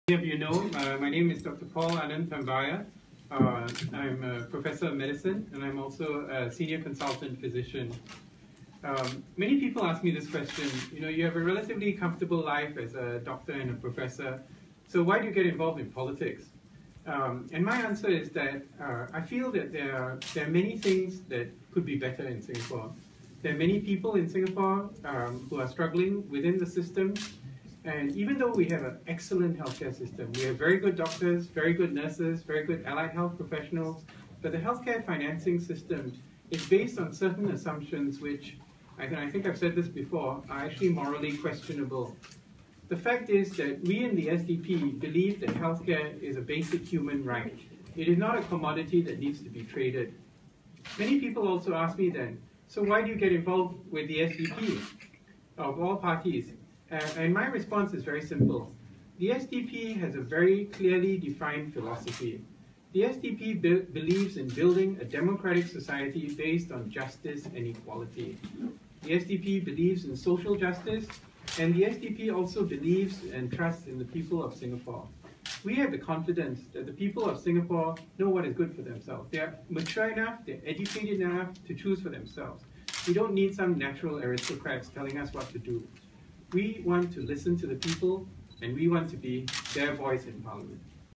SDP's Paul Tambyah giving his self-introduction
Singapore Democratic Party candidate Paul Tambyah giving a self-introduction on Aug 26, 2015. Video shot by Pearl Lee.